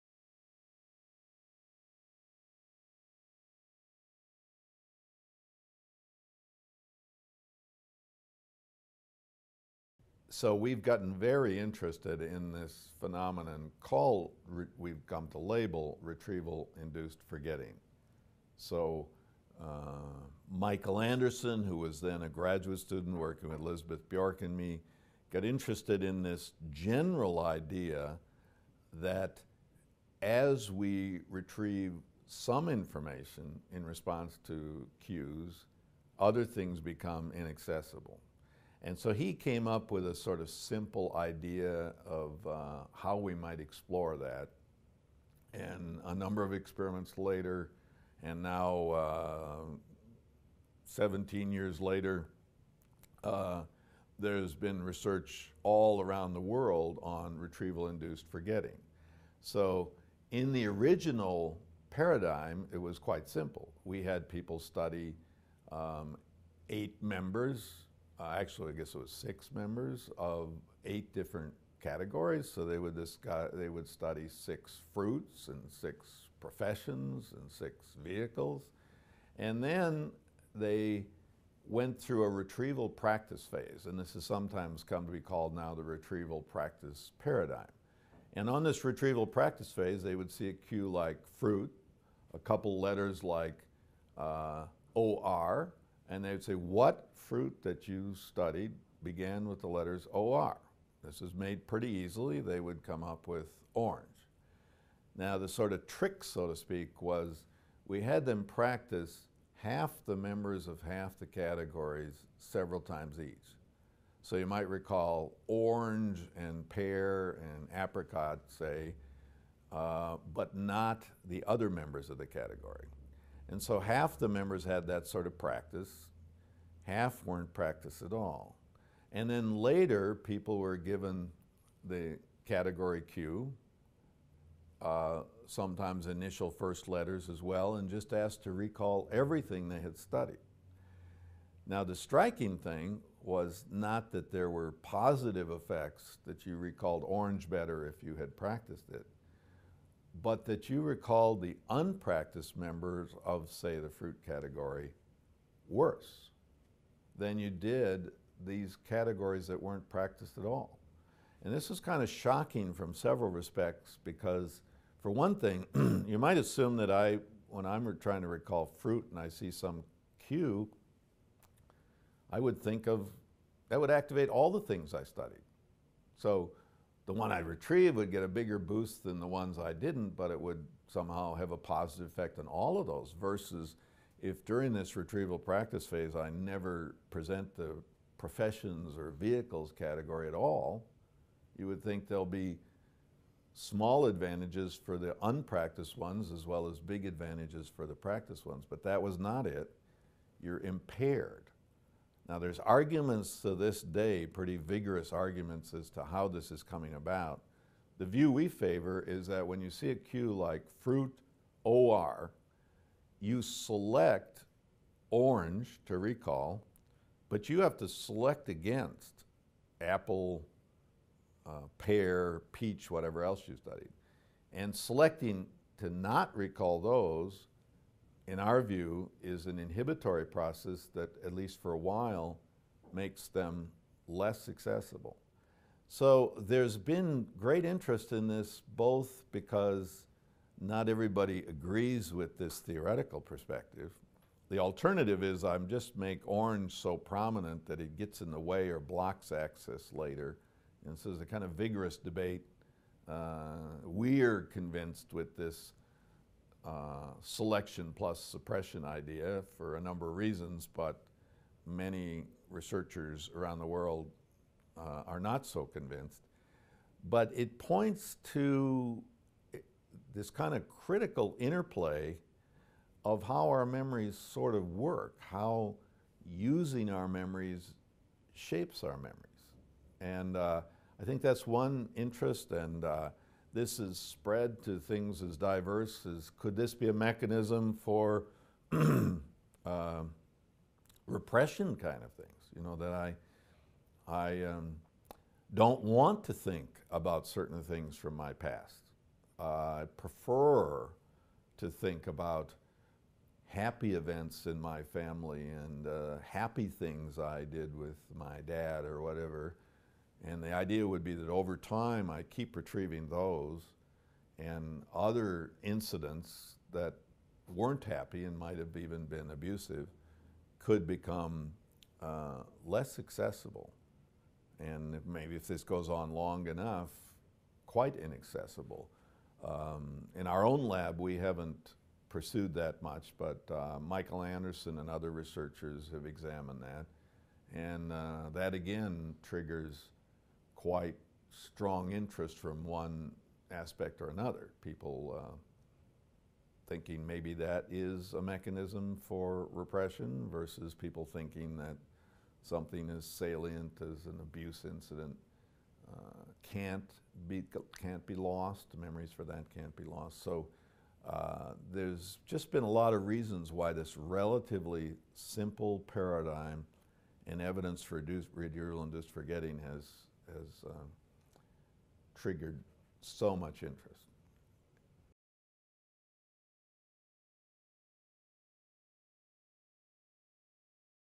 robert bjork - retrieval induced forgetting
When retrieving information from memory the retrieved memory will be strengthened.  However, competing memories will be less accessible afterwards.  This implies that remembering doesn't only produce positive effects for the remembered facts or events, but it might also lead to forgetting of other, related things in memory.